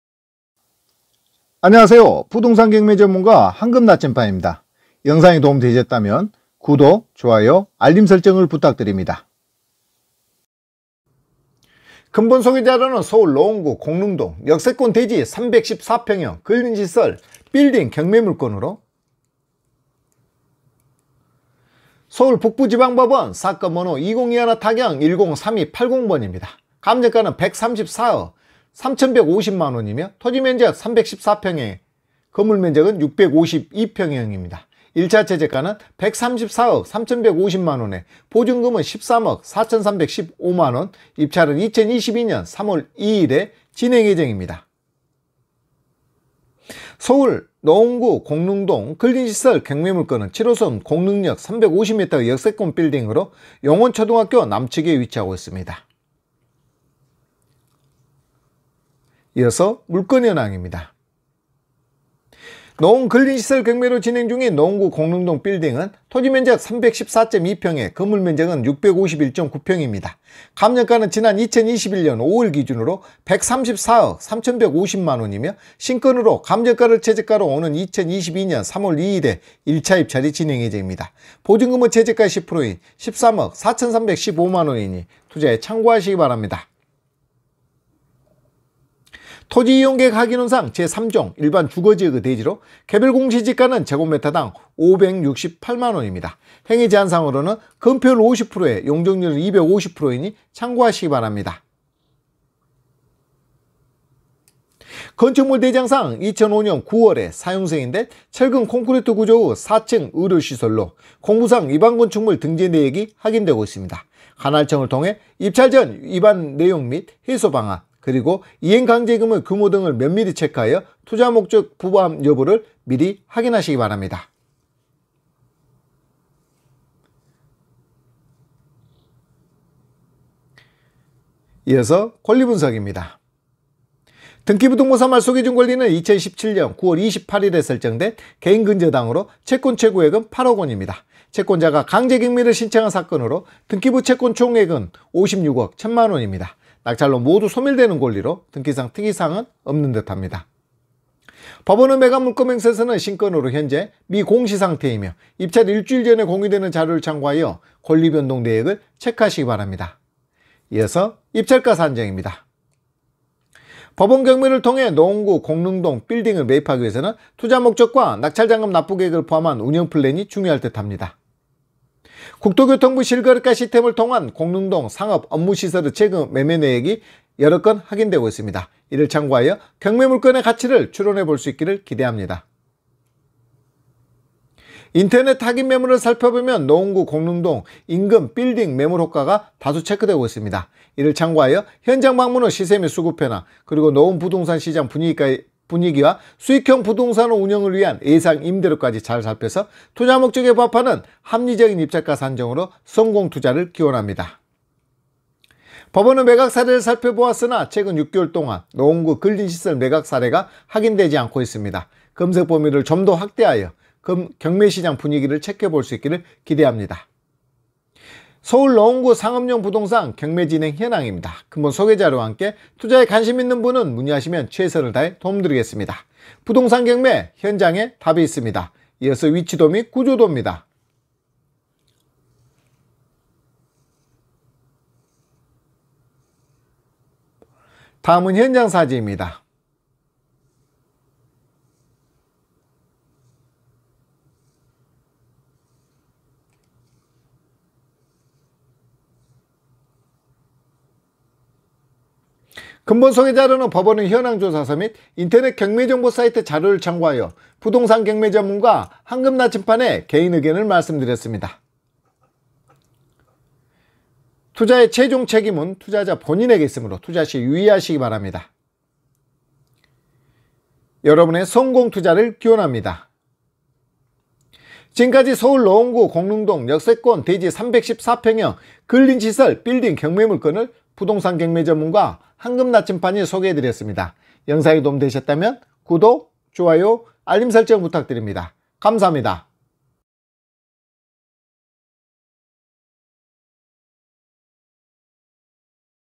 서울 노원구 공릉동 역세권 대지 314평형 근린시설 빌딩 경매
[ 서울 북부 지방법원 사건번호 2021 타경 103280 ]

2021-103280
감정가 134억 3150만 원
토지면적 314평형
건물면적 652평형
1차 최저가 134억 3150만 원
보증금 13억 4315만 원

※ 자세한 경매정보는 아래 주소를 클릭하여 블로그를 통해 확인 바랍니다.